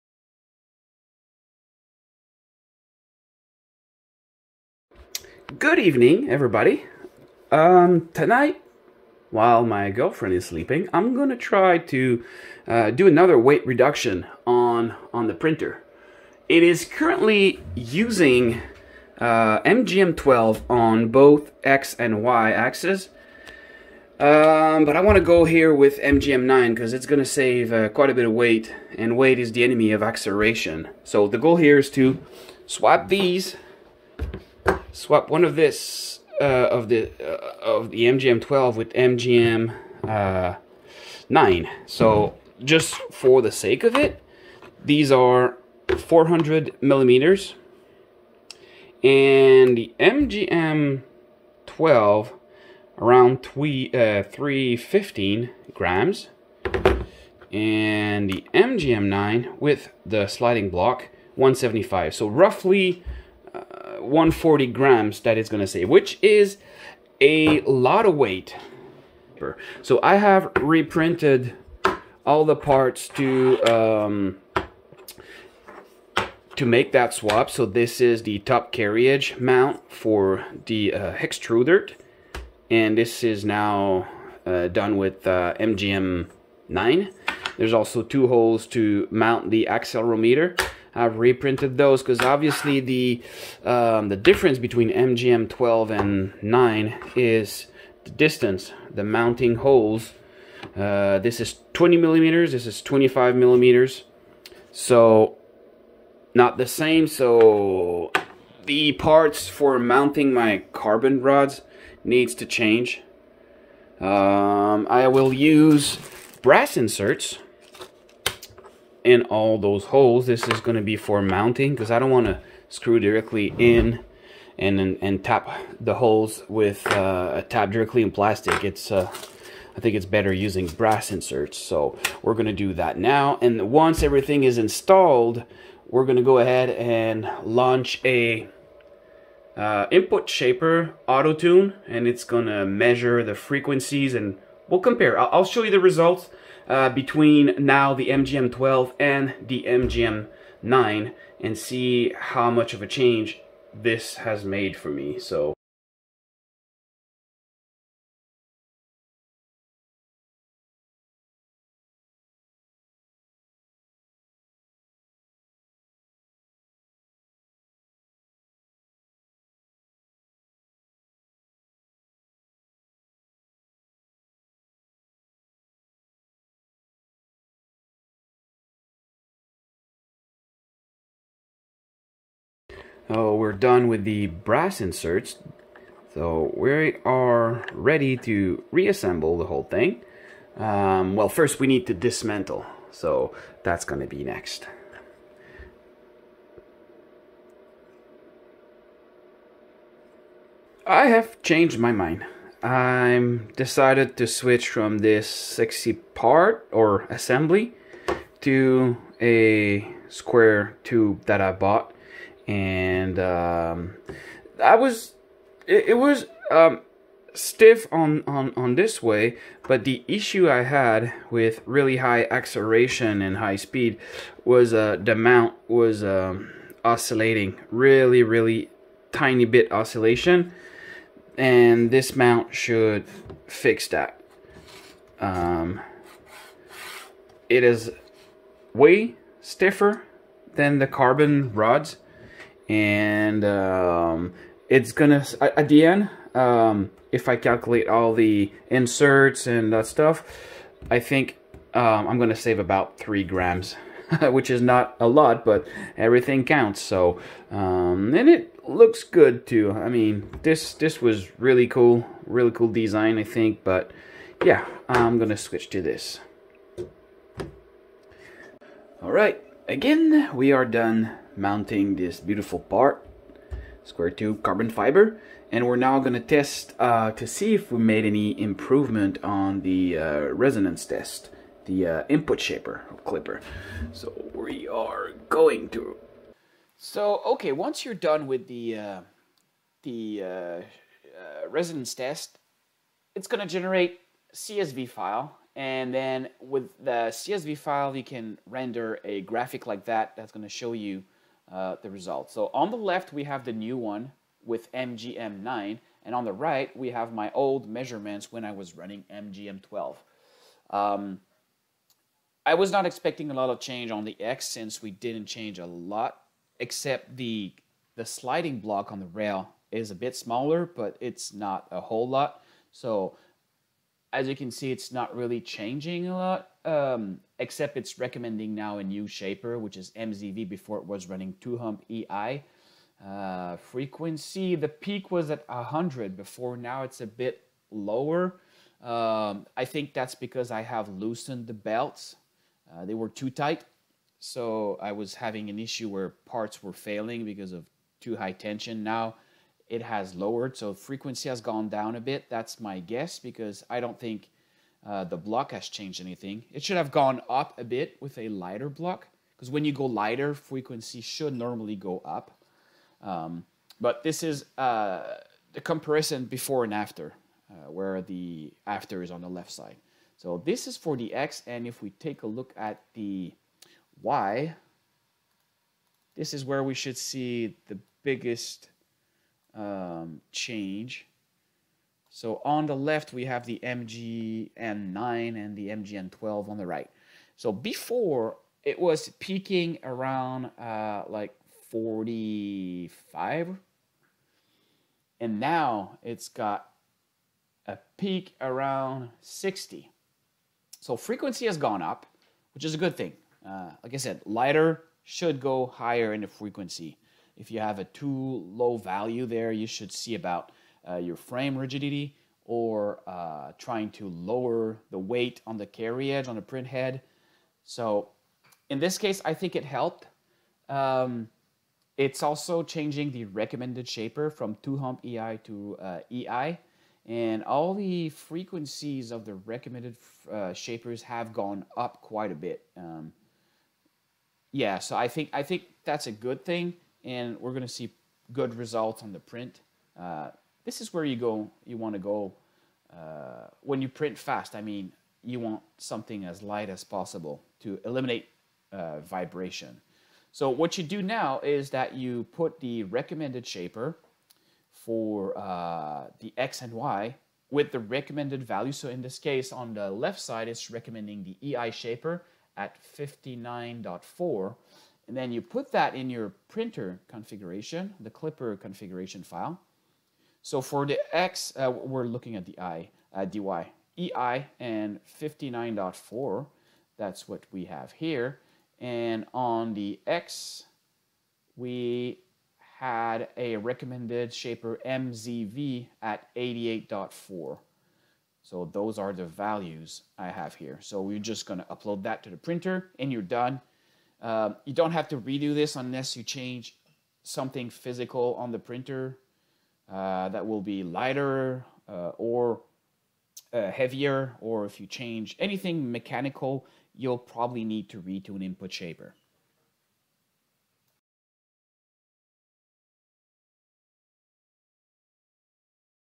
Weight reduction MGN12 to MGN9 on X
Removing 140G. Retuning with Input_shaper.